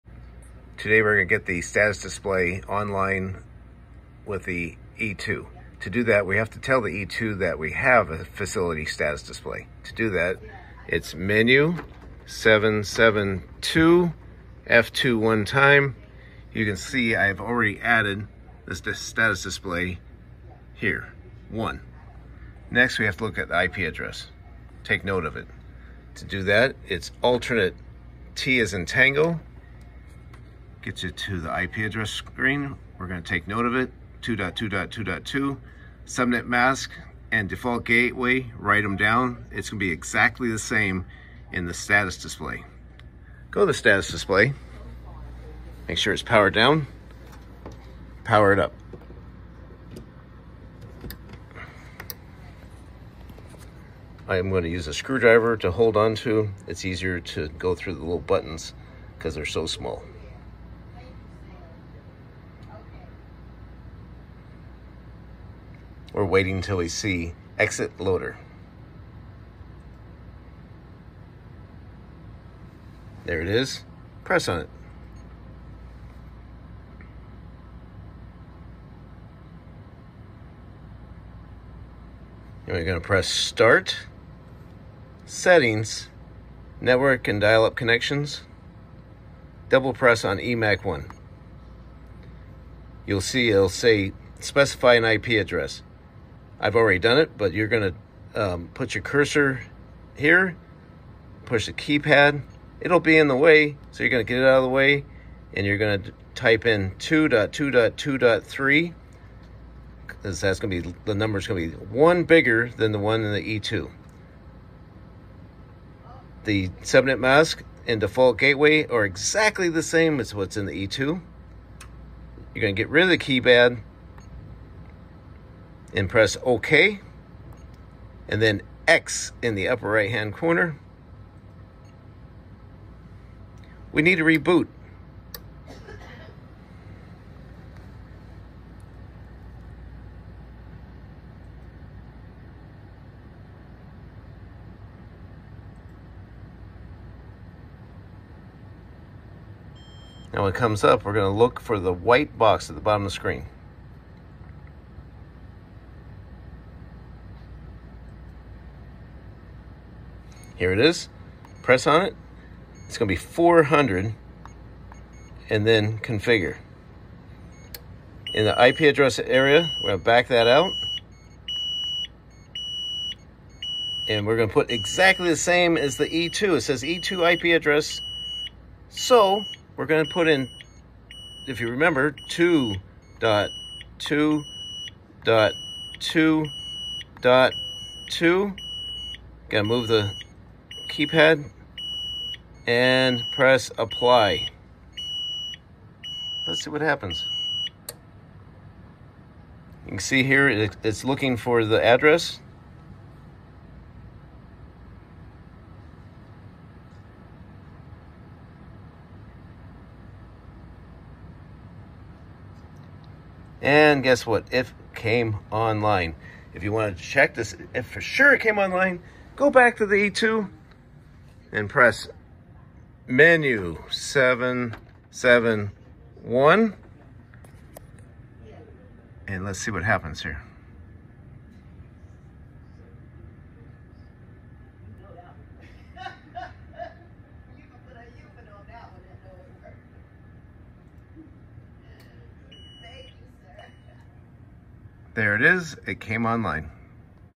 How to get the FSD and E2 online, facility status display. Emerson. Copeland.
This video will help you get your FSD and E2 online with each other through ethernet.